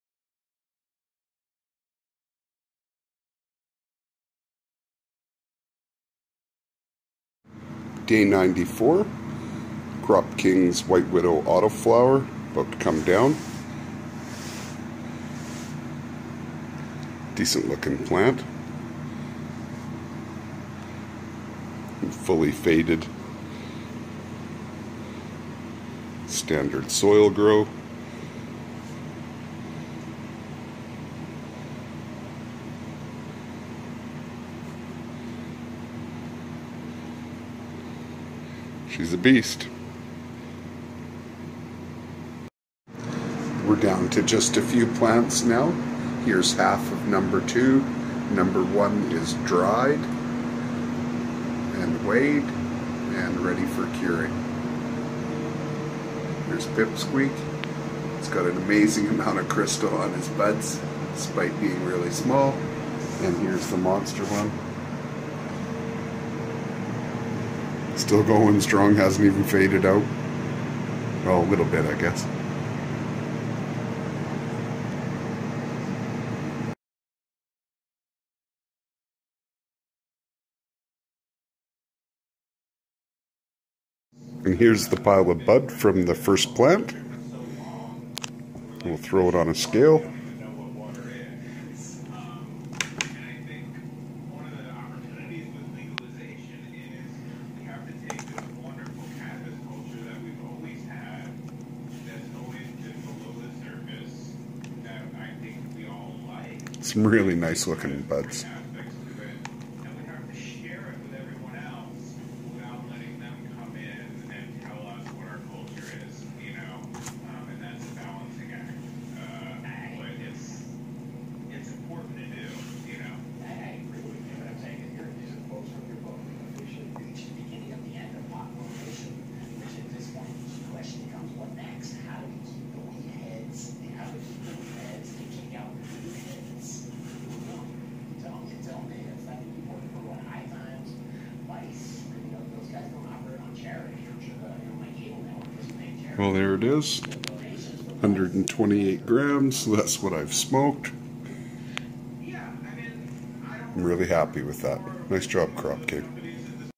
Harvesting Some Crop King Seeds White Widow Auto Flowers - Day 94
Video of My Plants and then the Harvest. Documenting My Progress Cultivating My Cannabis. Cutting Down and Totalling some of my previous dried Cannabis from the White Widow Auto Flowers.

Adults only: Follow your local laws governing the cultivation of Cannabis.
Legal Canadian Medical Cannabis Cultivation Under the ACMPR Permit.
ACMPR =Access to Cannabis for Medical Purposes Regulations